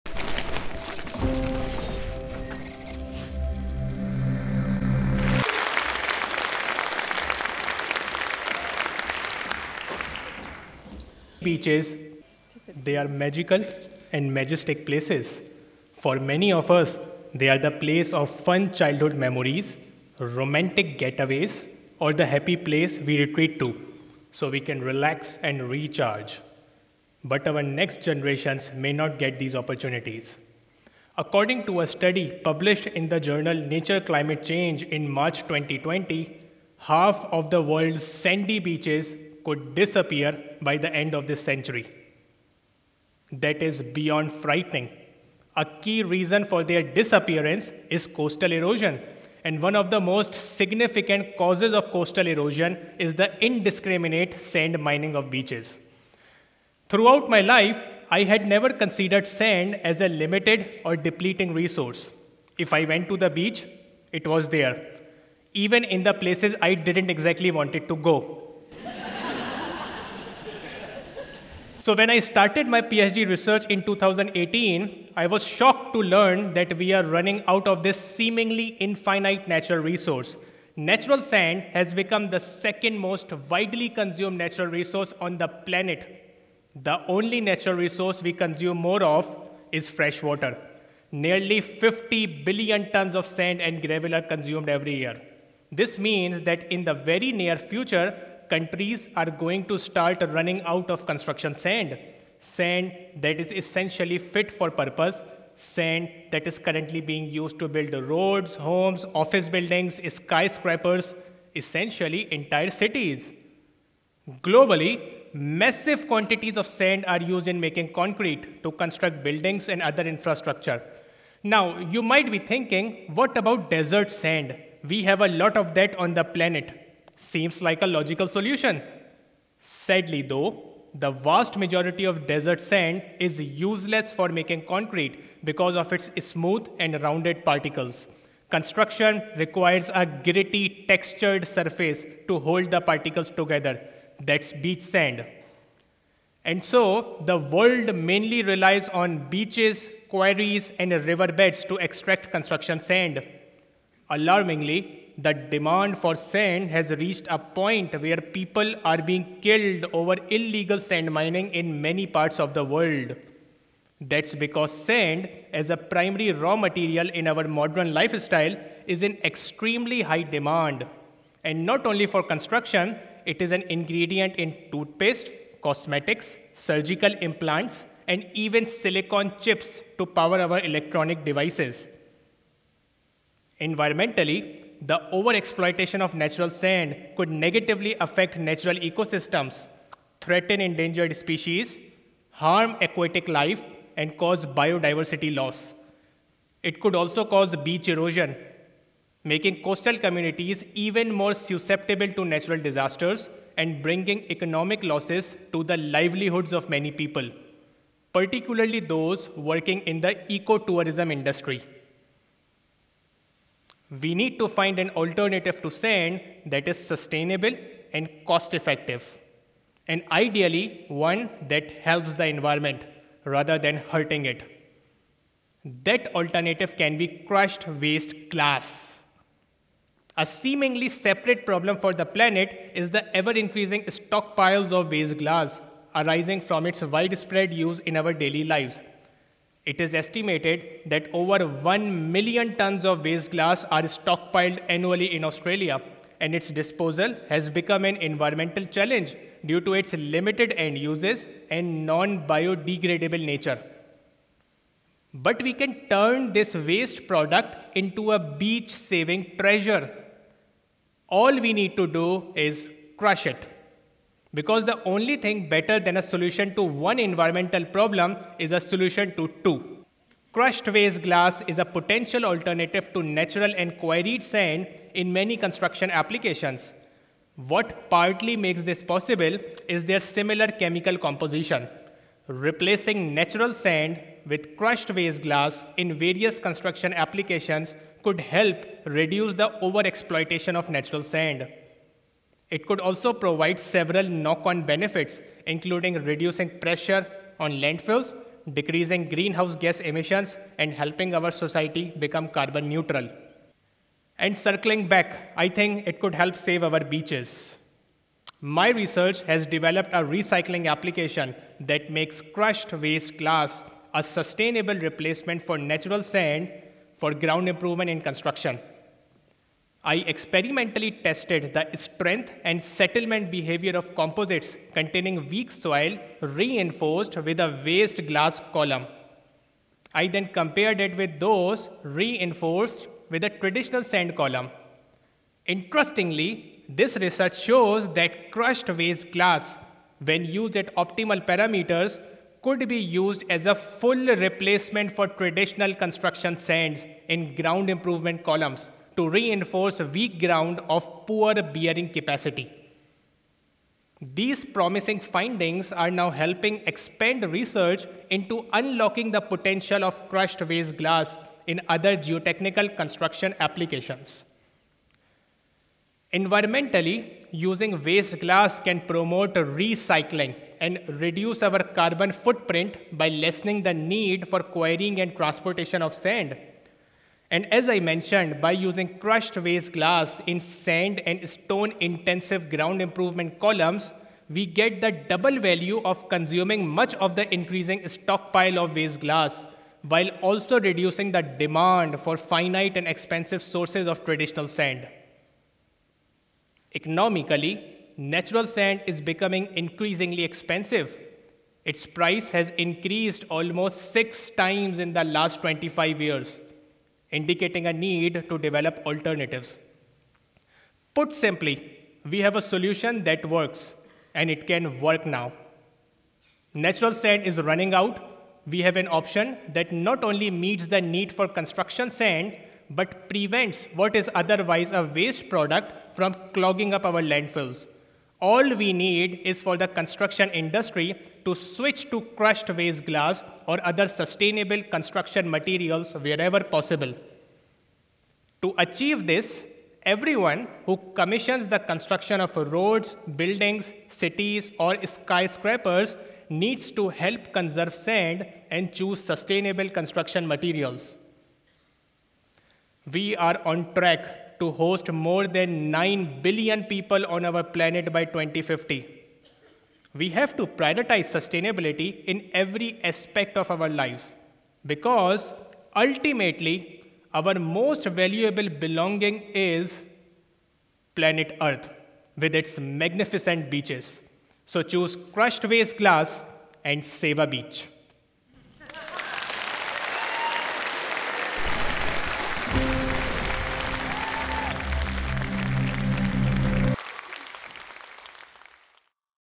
How to reduce sand mining and save beaches using crushed waste glass | Danish Kazmi | TEDxBrisbane
According to a recent study, half of the world’s sandy beaches could disappear by the end of this century. A key reason for their disappearance is coastal erosion. One of the most significant causes of coastal erosion is the indiscriminate sand mining of beaches. Civil and geotechnical engineer and researcher Dr Danish Kazmi is championing the use of crushed waste glass as a sustainable alternative. Adopting this alternative materials approach would offer a dual benefit of helping to reduce excessive sand mining while addressing the environmental problem of fast-growing stockpiles of non-biodegradable waste glass, supporting the transition to a circular economy. Dr Danish Kazmi is a civil and geotechnical engineer, researcher and award-winning science communicator. He is a Geotechnical Engineer at GHD in Australia and holds an Adjunct Lecturer position at The University of Queensland where he is involved in leading-edge research to develop transformative civil and geotechnical engineering solutions emerging from cross-institutional and university-industry collaborations.

His research focuses on developing sustainable geomaterials by recycling ever-increasing wastes as eco-friendly alternatives to depleting natural sand and aggregates in geotechnical construction, helping to reduce the over-exploitation of natural sand and supporting the transition to a circular economy.

His research has been published in top-ranked scientific journals and magazines. He is an invited peer-reviewer for leading international journals, a UQ Global Change Scholar and Future Leader, certified Carbon Literate and an accredited Mental Health First Aider. This talk was given at a TEDx event using the TED conference format but independently organized by a local community.